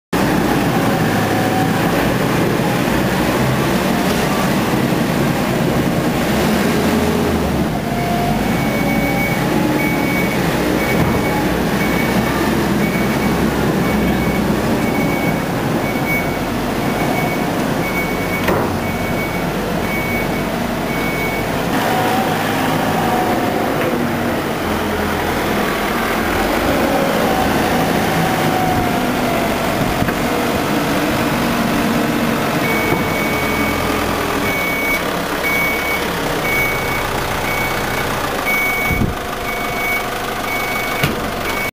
Merlo mucking out cattle shed
Merlo 26.6 mucking out in a cattle shed, and Fendt 818 and 14t trailer leading